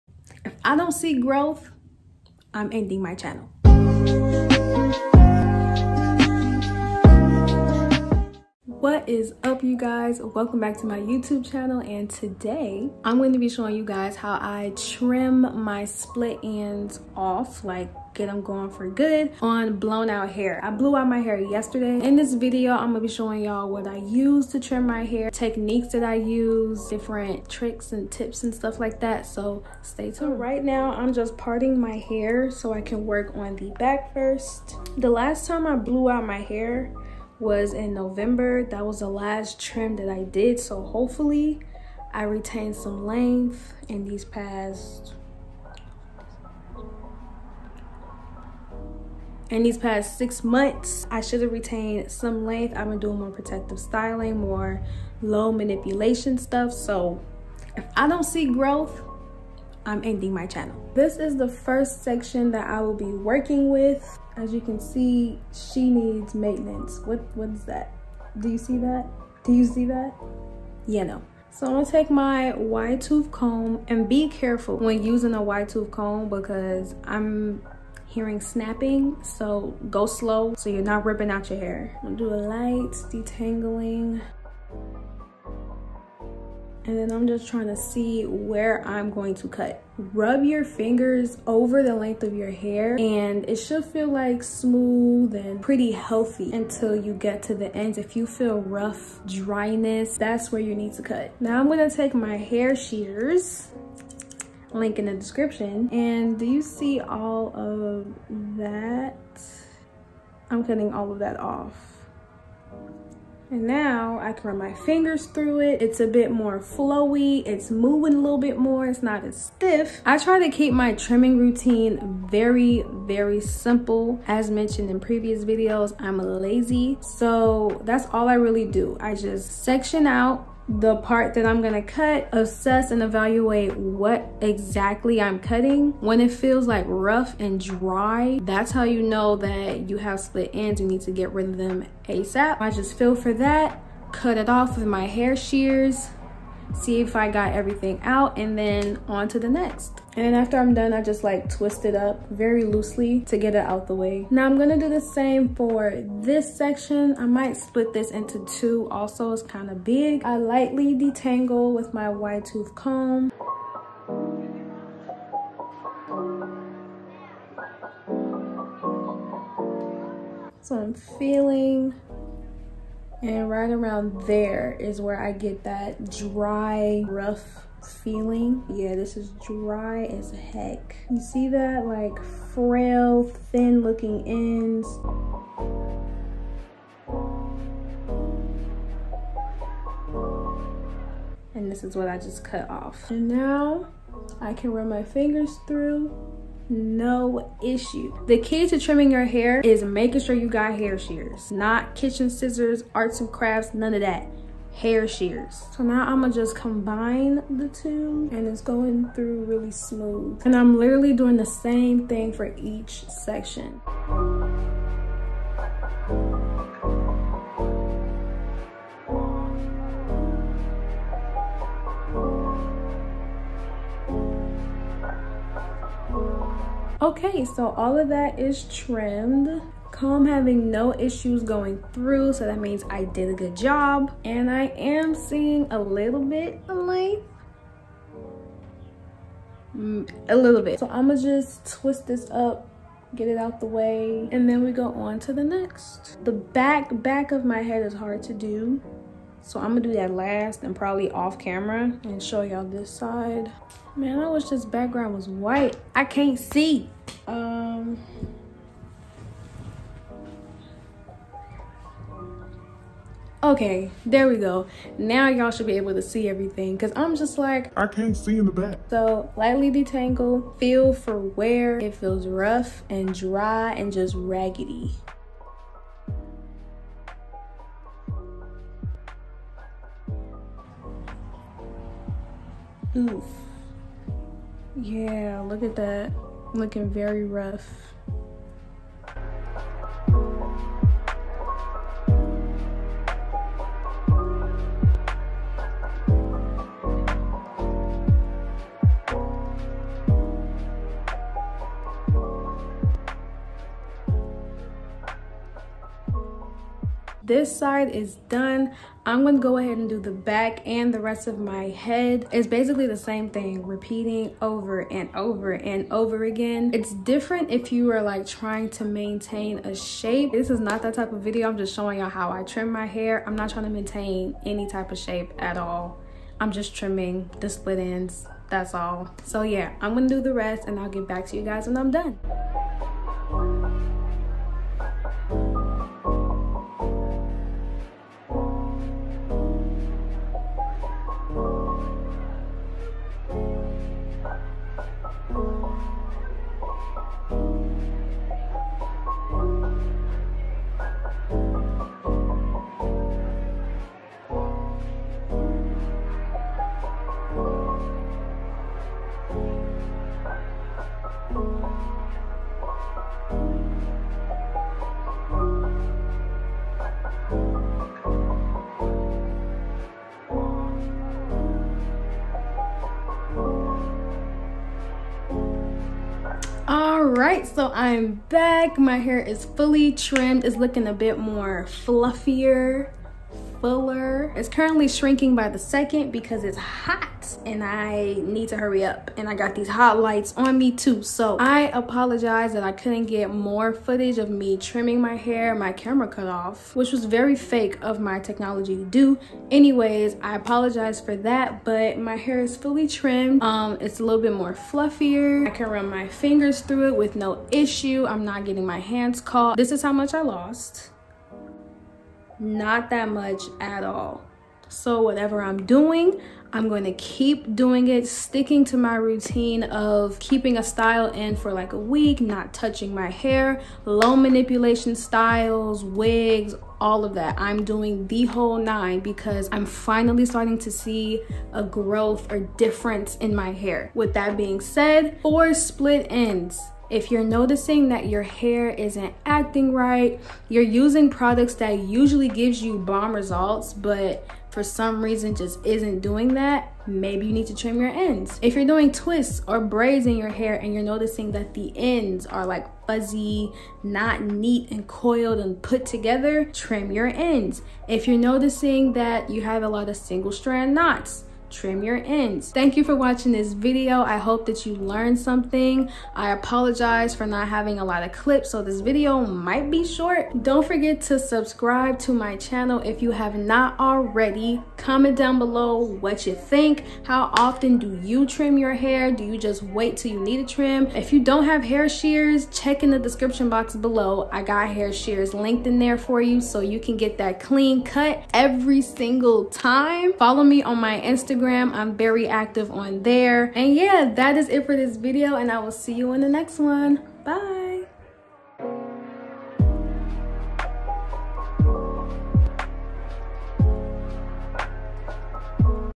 HOW I TRIM MY NATURAL HAIR! Cut off those SPLIT ENDS!!
Today I am trimming my split ends on blown-out hair at home. My last trim was November 2020 so it has been 6 months and I wonder if I retained any length?

Helpful Links from the video✨:
Hair Sheers